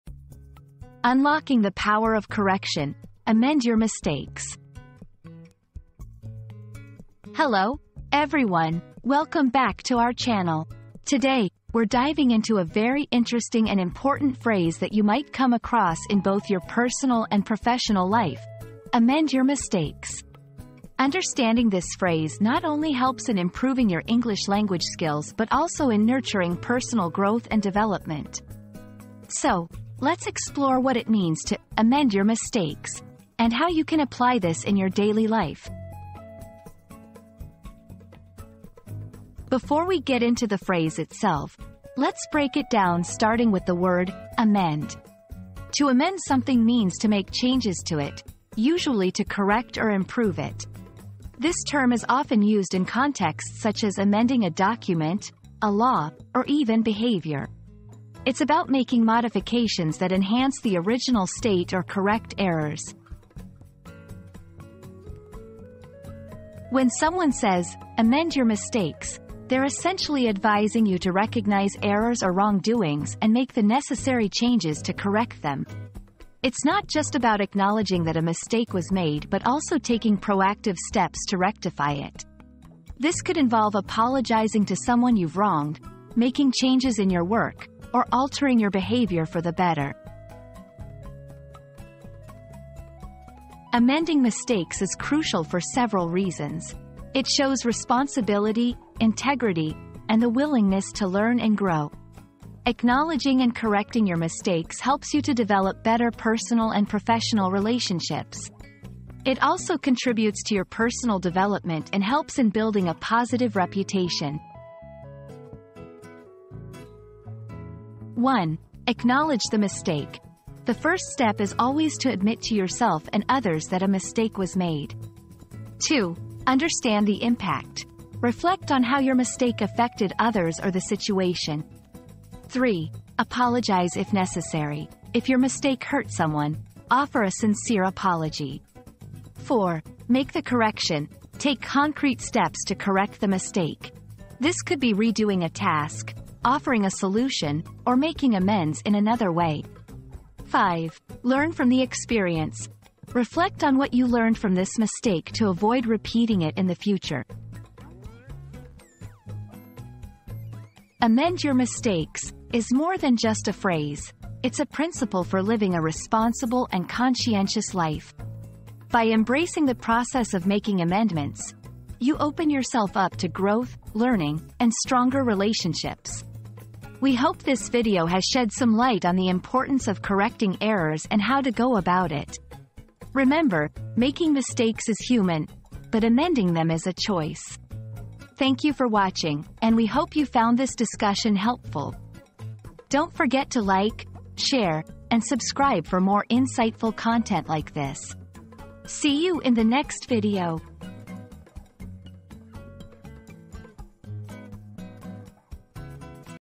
Unlocking the Power of Correction: "Amend Your Mistakes"
Amend Your Mistakes: Unlocking the Power of Correction • Discover the transformative power of correcting your mistakes and unlocking your full potential. Learn how to embrace growth and turn setbacks into opportunities for success.

00:00 • Introduction - Unlocking the Power of Correction: "Amend Your Mistakes"
00:38 • Understanding "Amend"
01:06 • What Does "Amend Your Mistakes" Mean?
01:33 • Why Is It Important?
01:57 • How to Amend Your Mistakes